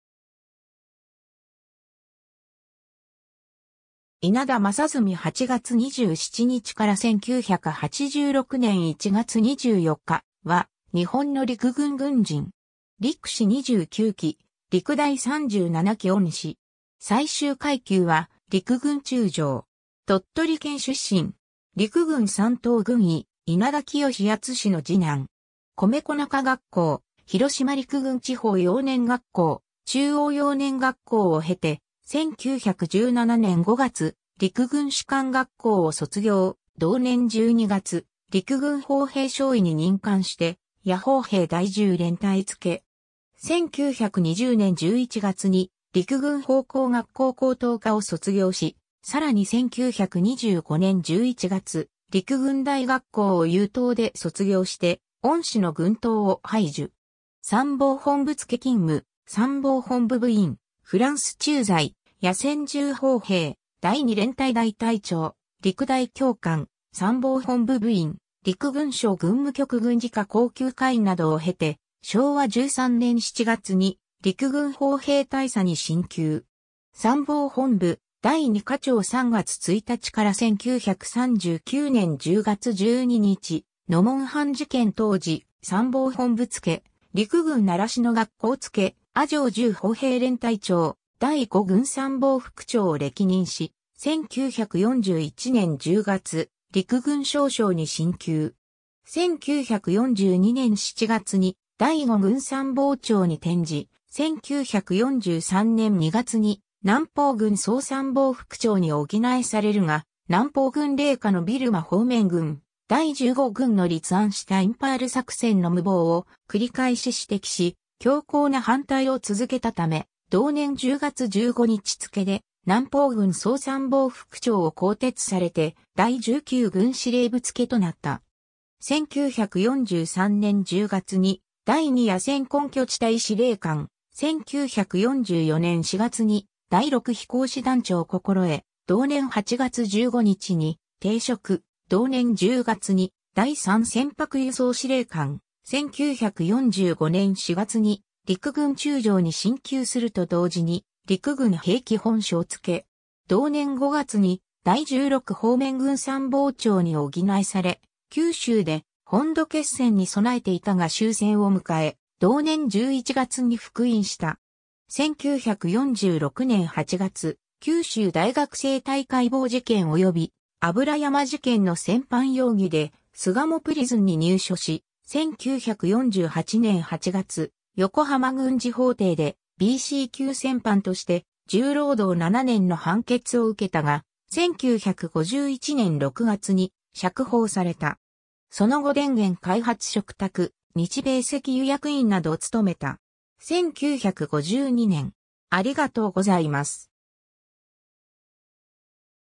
稲田正純
稲田 正純（いなだ まさずみ、1896年（明治29年）8月27日 - 1986年（昭和61年）1月24日）は、日本の陸軍軍人。
陸士29期・陸大37期恩賜。
最終階級は陸軍中将。
 鳥取県（日野町）出身。
陸軍三等軍医（少尉相当官。戦死）・稲田清淳の二男。
米子中学校（現在の米子東高校）、広島陸軍地方幼年学校、中央幼年学校を経て、1917年（大正6年）5月、陸軍士官学校（29期）を卒業、同年12月、陸軍砲兵少尉に任官して野砲兵第10連隊付。
1920年（大正9年）11月に陸軍砲工学校高等科を卒業し、さらに1925年（大正14年）11月、陸軍大学校（37期）を優等（3席）で卒業して恩賜の軍刀を拝受。
 参謀本部付勤務、参謀本部部員、フランス駐在（フランス陸軍大学校卒業）、野戦重砲兵第2連隊大隊長、陸大教官、参謀本部部員（防衛課）、陸軍省軍務局軍事課高級課員などを経て昭和13年7月に陸軍砲兵大佐に進級。
参謀本部第2課長（作戦課長）（1938年（昭和13年）3月1日 - 1939年（昭和14年）10月12日、ノモンハン事件当時）、参謀本部付、陸軍習志野学校付、阿城重砲兵連隊長、第5軍参謀副長を歴任し、1941年（昭和16年）10月、陸軍少将に進級。
 1942年（昭和17年）7月に第5軍参謀長に転じ、1943年（昭和18年）2月に南方軍総参謀副長に補されるが、南方軍隷下の緬甸方面軍・第15軍の立案したインパール作戦の無謀を繰り返し指摘し、強硬な反対を続けたため、同年10月15日付で南方軍総参謀副長を更迭されて第19軍司令部付となった 。
1943年（昭和18年）10月に第2野戦根拠地隊司令官、1944年（昭和19年）4月に第6飛行師団長心得、同年8月15日に停職、同年10月に第3船舶輸送司令官、1945年（昭和20年）4月に陸軍中将に進級すると同時に陸軍兵器本廠付。
同年5月に第16方面軍参謀長に補され、九州で本土決戦に備えていたが終戦を迎え、同年11月に復員した。
 1946年（昭和21年）8月、九州大学生体解剖事件及び油山事件（油山米兵捕虜斬首事件）の戦犯容疑で巣鴨プリズンに入所し、1948年（昭和23年）8月、横浜軍事法廷でBC級戦犯として重労働7年の判決を受けたが、1951年（昭和26年）6月に釈放された。
その後電源開発嘱託、日米石油役員など務めた。
 1952年